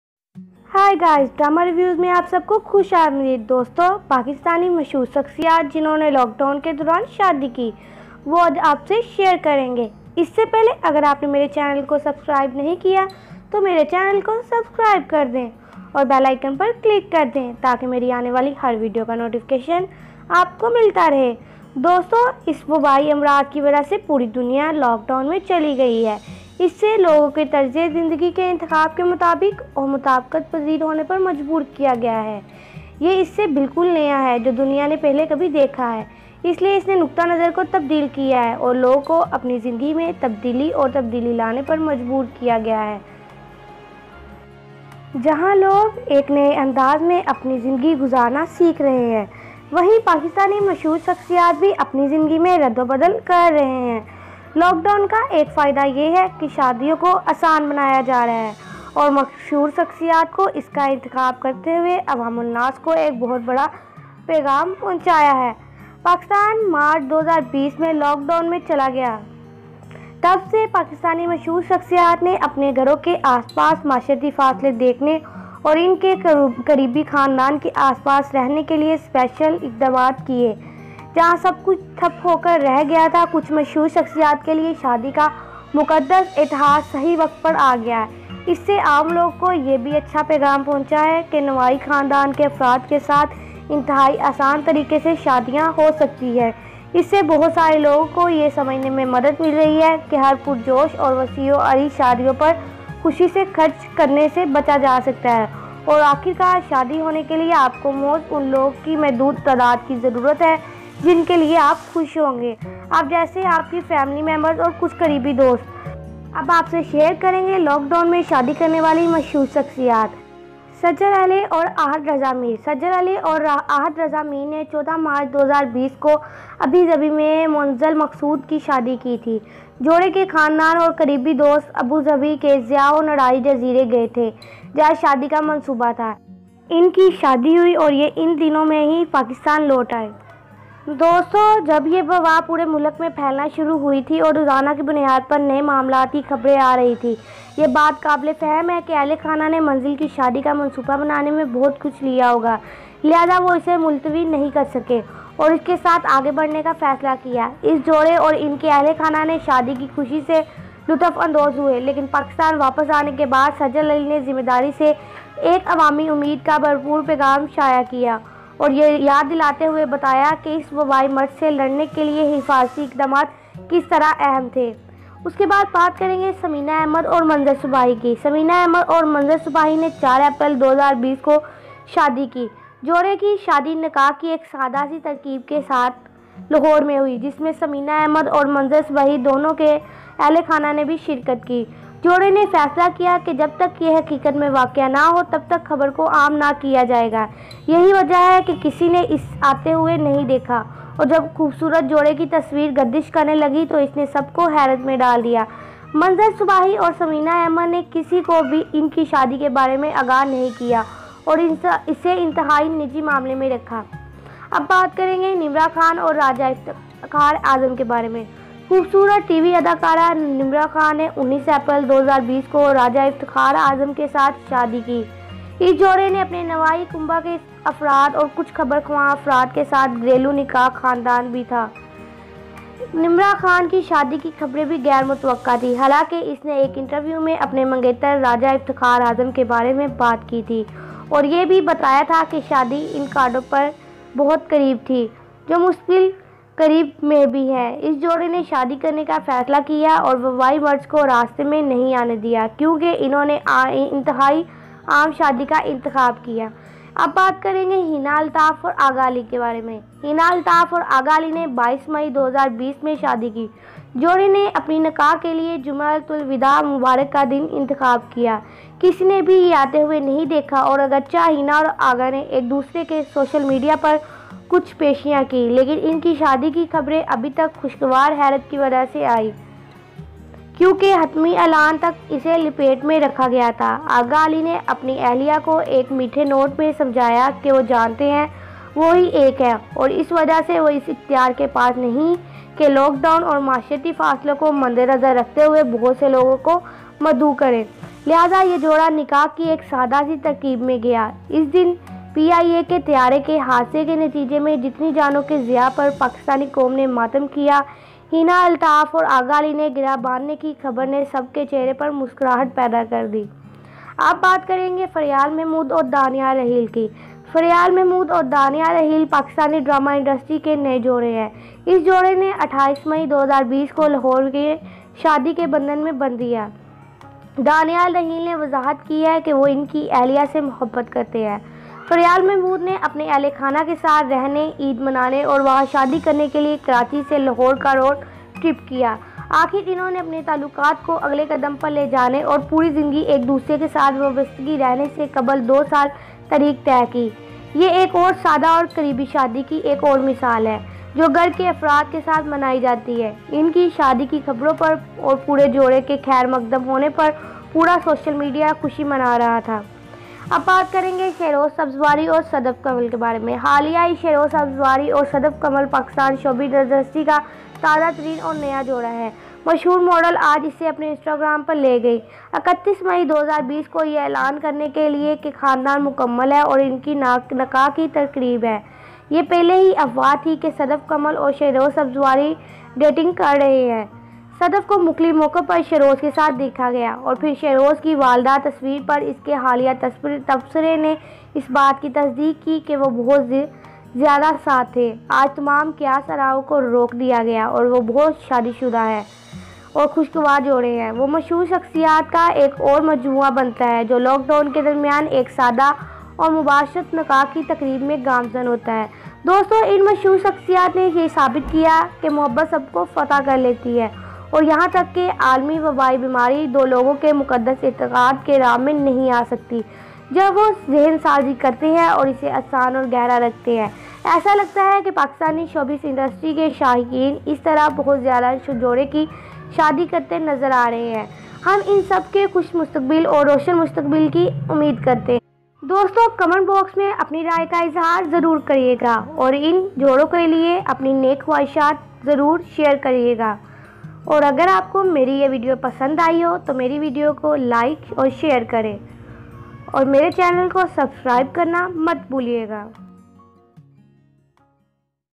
Pakistani Celebrities Who Got Married During Lockdown
Here are the Pakistani celebrities who got married in the Lockdown: